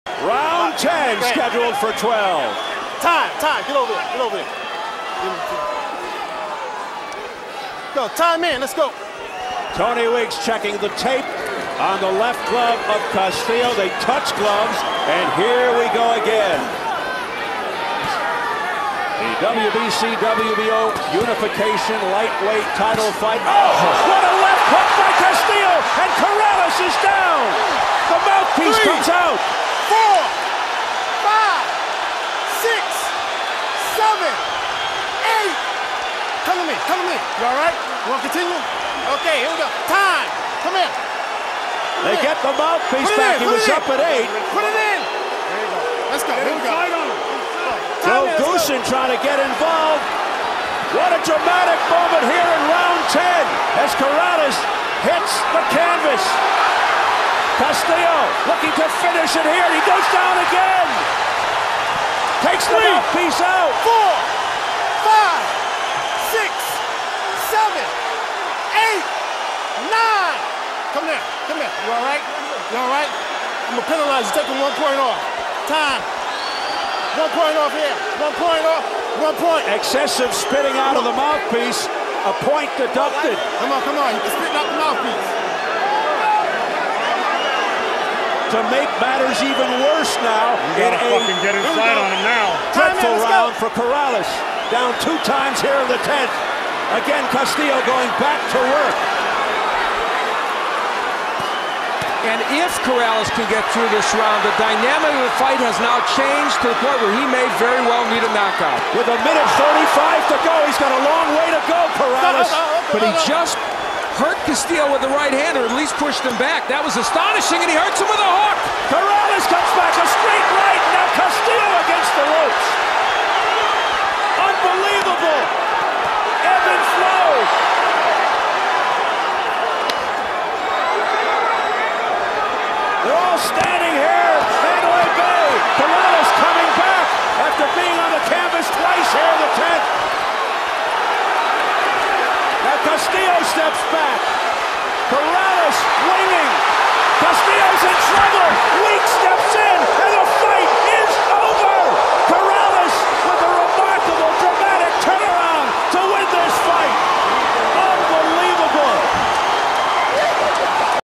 Corrales vs. Castillo I - Round 10 [HD]
2005 Ring Magazine Round of the Year from Mandalay Bay, Las Vegas.  Diego Corrales vs. Jose Luis Castillo.  07/05/2005